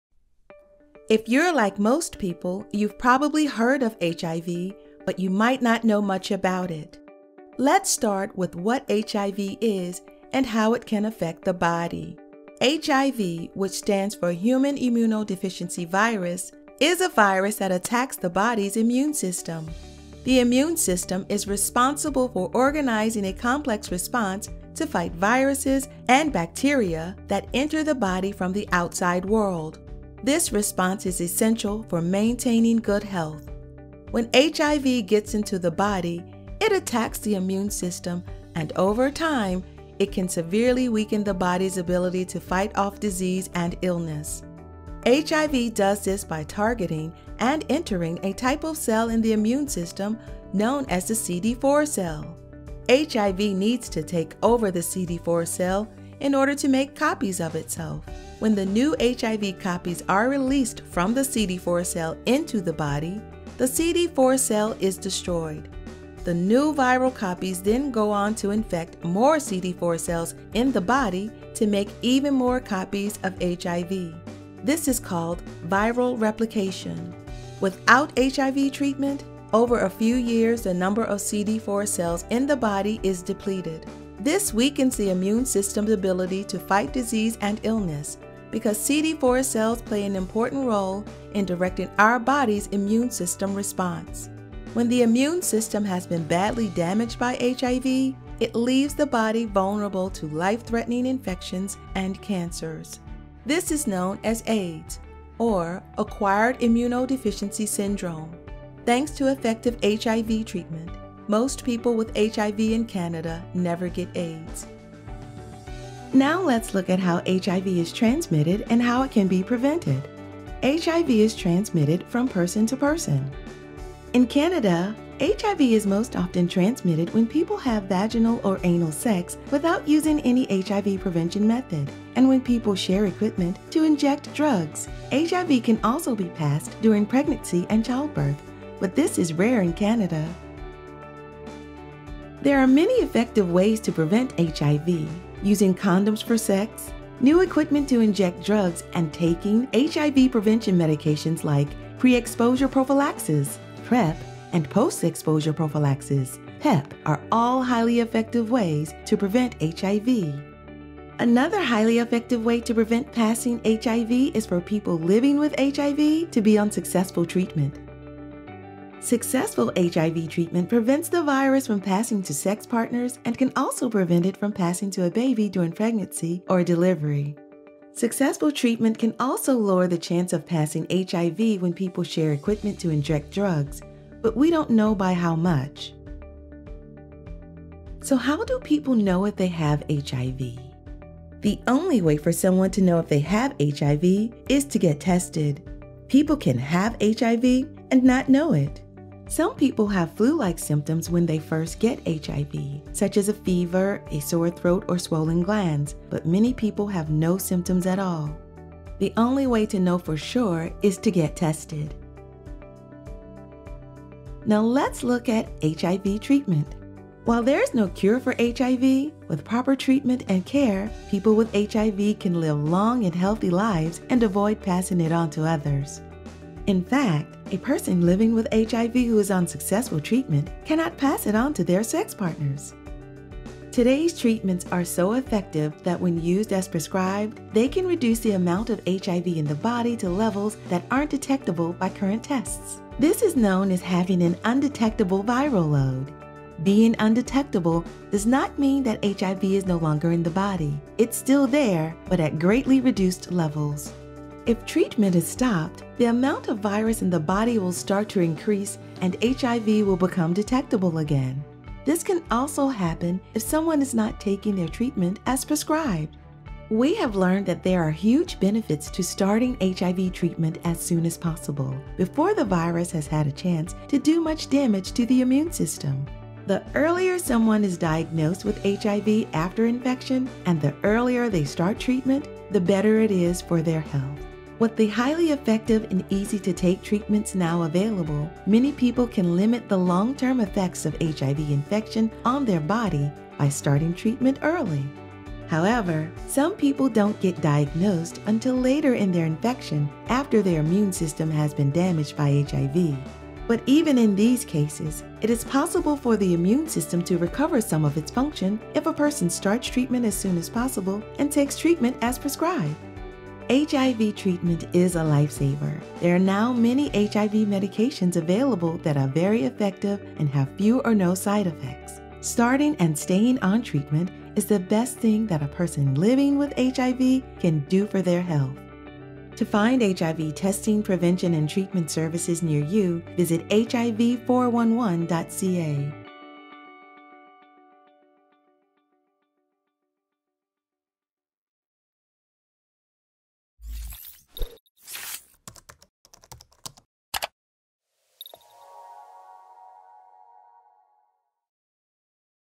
HIV Basics
This short video includes basic information about HIV, such as how HIV affects the body, how it is passed, ways to prevent transmission, testing and treatment.